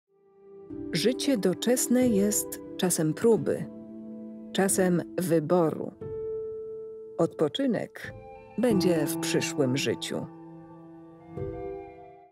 Życie doczesne jest czasem próby, czasem ... | Ksiądz Blachnicki
,,Życie doczesne jest czasem próby, czasem wyboru. Odpoczynek będzie w przyszłym życiu.'' - Ksiądz Franciszek Blachnicki